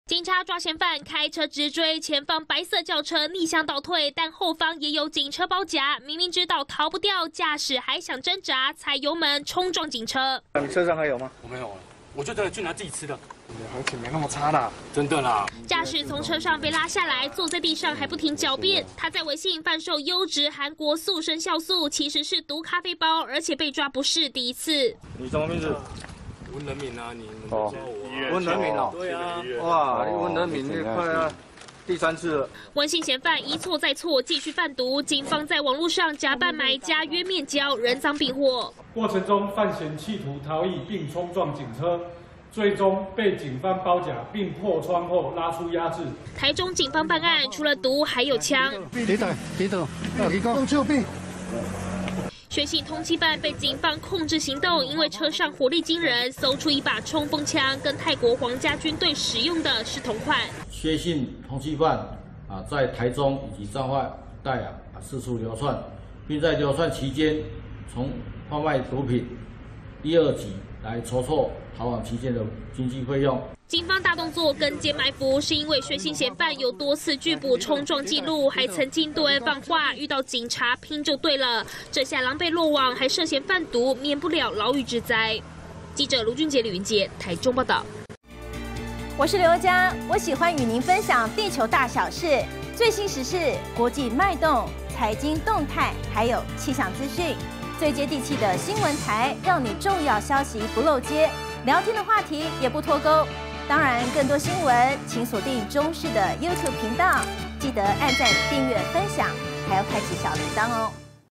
毒咖啡包偽裝瘦身酵素 藥頭面交被逮喊"你抓過我"│中視新聞 20231211
台中有男子，在網路上販賣"韓國塑身酵素"，其實是毒咖啡包，警方假扮賣家跟嫌犯約面交，他竟然拒檢衝撞警車，被拖下車時，還跟警方說「你抓過我啊」，狡辯這次不是賣毒，只是自己用，但人贓並獲，還是被移送法辦。